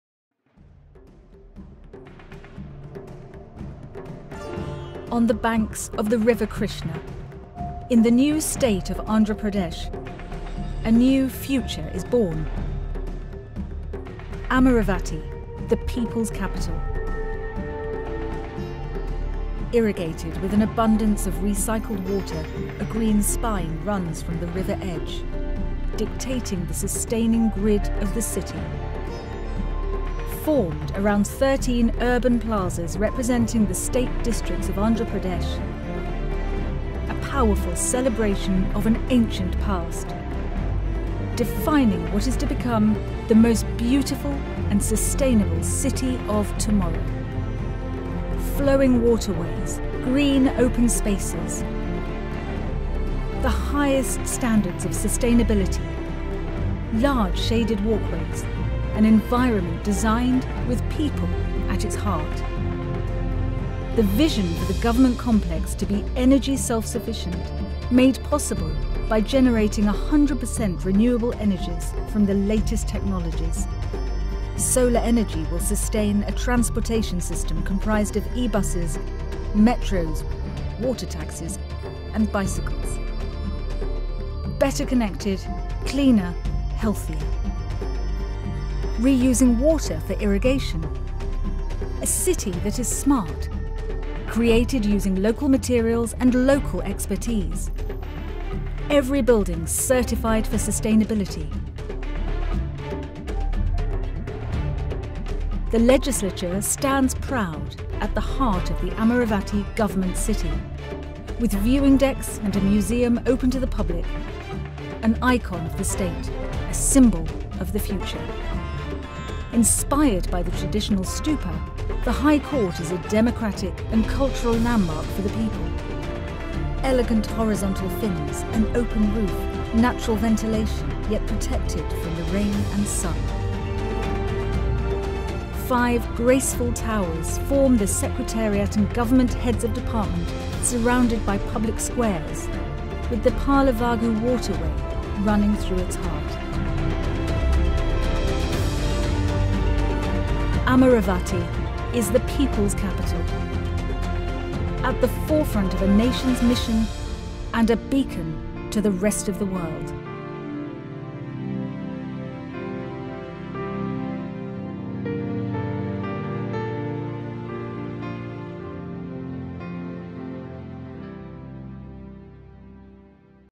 Amaravati Government Complex masterplan by Foster + Partners
Situated on the banks of the River Krishna, the new city of Amaravati will be one of the most sustainable in the world. Measuring 1475 acres, the governmental complex occupies the heart of the city, defined by a strong urban grid that structures the city.

Inspired by Lutyen’s New Delhi and New York’s Central Park, a clearly defined green spine runs through its length, providing the foundation of the masterplan’s environmental strategy, where at least 60% of the area is occupied by greenery or water.

The city has been designed to the highest standards of sustainability, with the latest technologies that are currently being developed in India.
(Video courtesy of Foster+Partners)